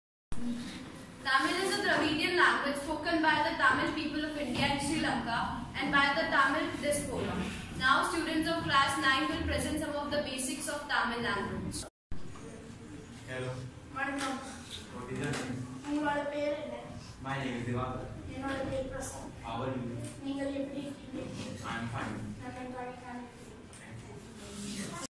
tamil jainhappyschool
Title of the video: tamil jainhappyschool
Name of the School: Jain Happy School
Address: Jain Happy School, Sector-II,  DIZ area, Gole Market, New Delhi-110001
State: Delhi

Bhasha Sangam, an initiative of Ministry of Human Resource Development,Dept. of School Education and Literacy, Govt. of India under Ek Bharat Shrestha Bharat programme is meant for schools and educational institutions to provide multilingual exposure to students in Indian languages listed in Schedule VIII of the
The students of Jain Happy School took initiative and encouraged to learn few lines in the different constitutional languages.
The students of Jain Happy School depicts the basics of Tamil language.